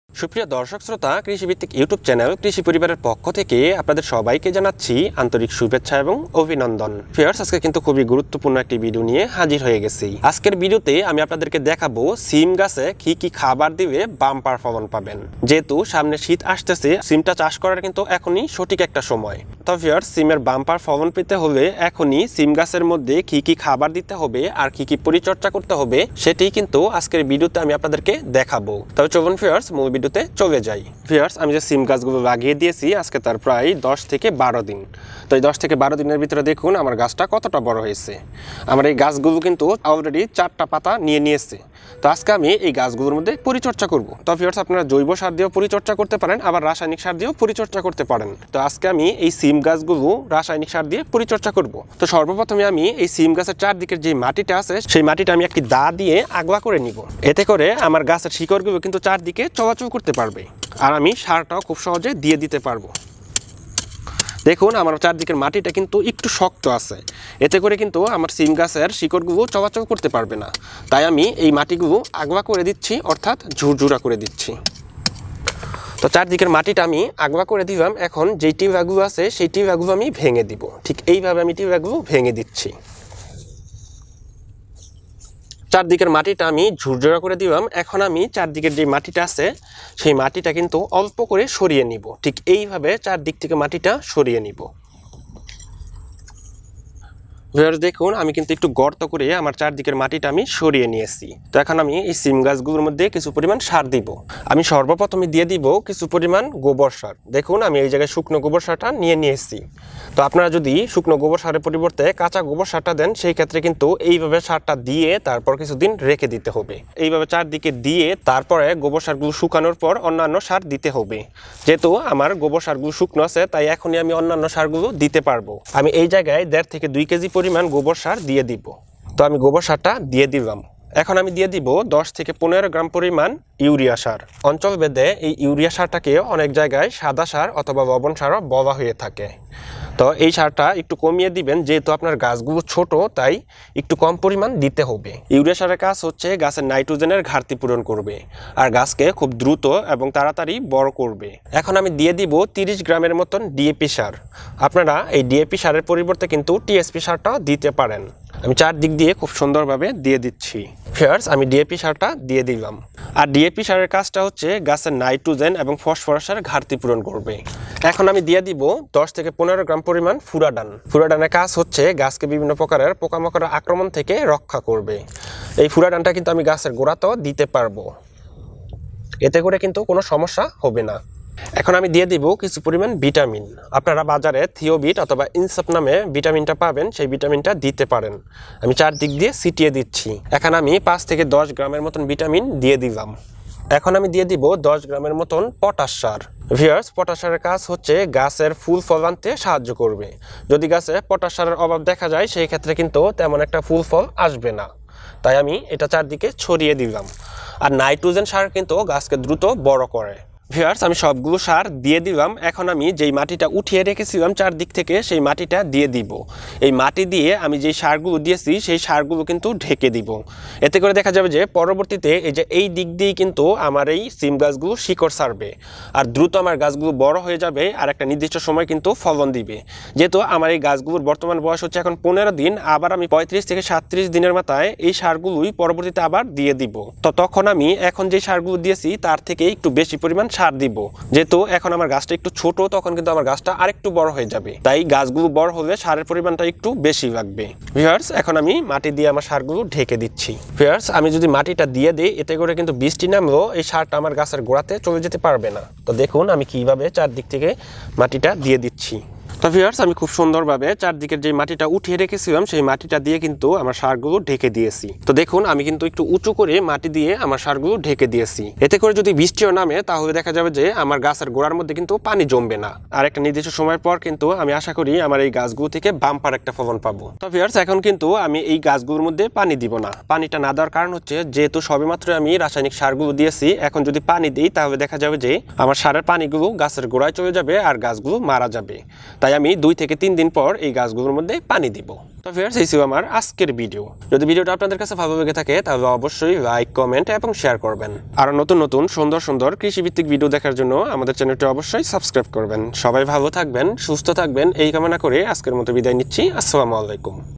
শিম গাছে কি কি খাবার দিলে বাম্পার ফলন পাবেন | শিম গাছের পরিচর্যা | Bean Cultivation
আজকে আমি আপনাদের দেখাবো শিম গাছে কি কি খাবার দিলে বাম্পার ফলন পাবেন।

যেহেতু সামনে শীত আসছে তাই শীম চাষ করার এখনি সঠিক সময়।

শিম বিজ লাগানোর আগে ভাল জাতের বীজ  সংগ্রহ করতে হবে।

হাইব্রিড জাতের শিম চাষ করতে পারলে সব থেকে লাভমান হওয়া যাবে।